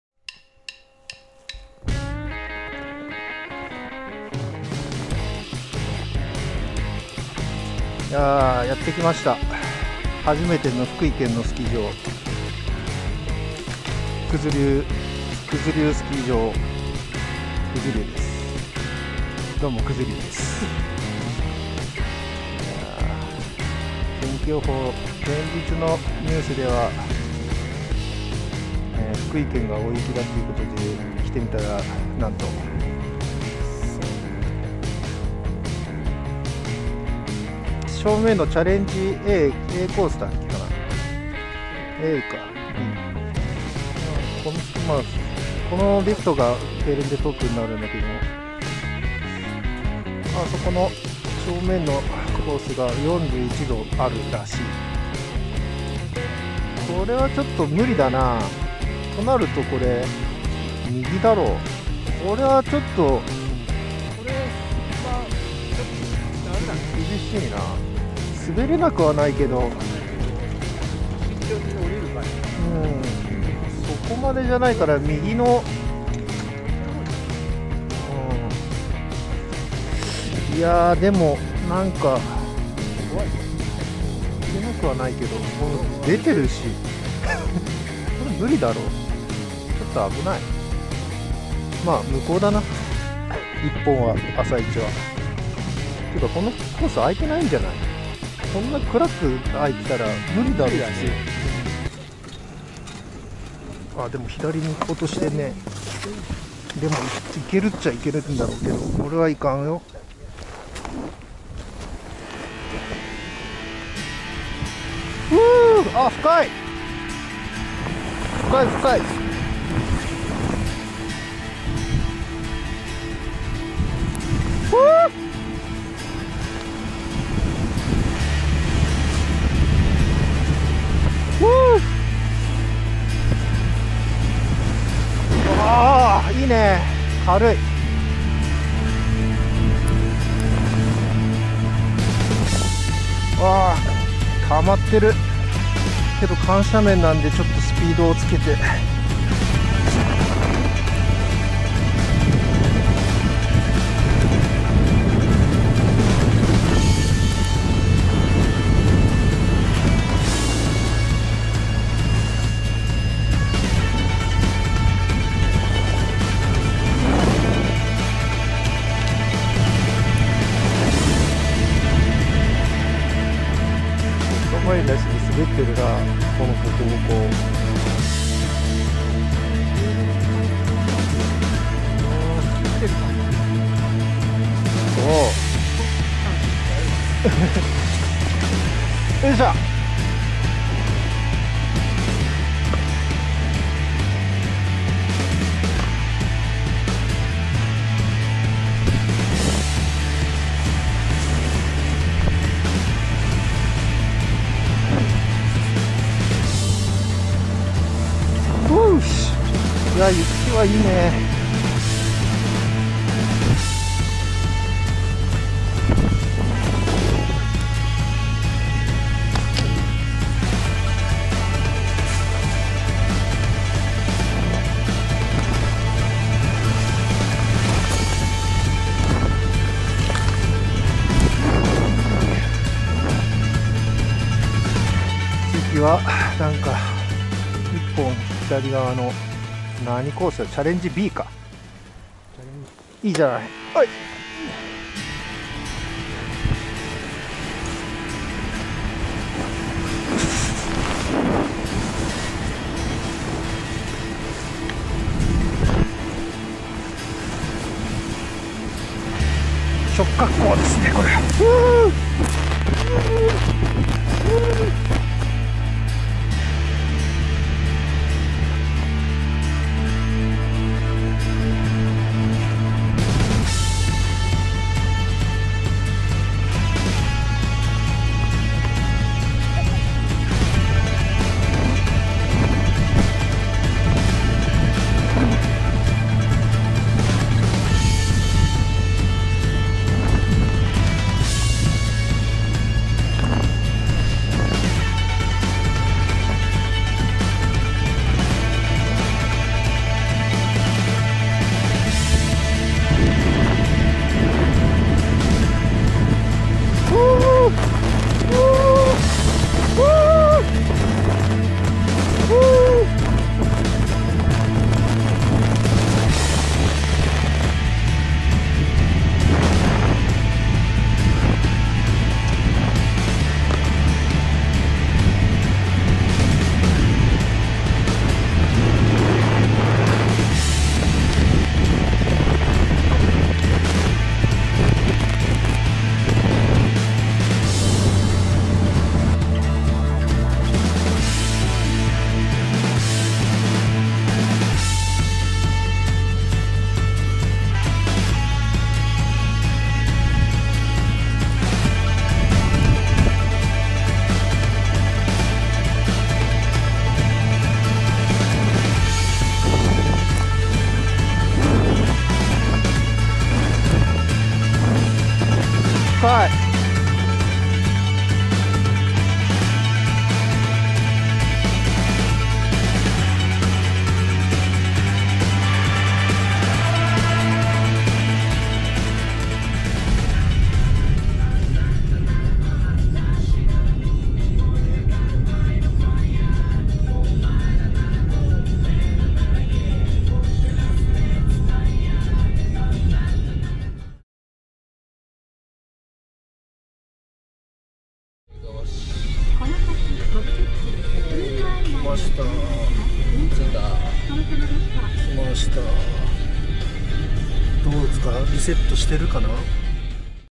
【SI-BA-TV 第15回】1月13日 九頭竜スキー場
1月13日は九頭竜スキー場へ
福井県が大雪ということで初めて福井のスキー場へ行ってきました。
気温も低く雪は今シーズン一番軽かったと思います。
ただ、コースが短く斜度も足りず少し残念な感じもしました。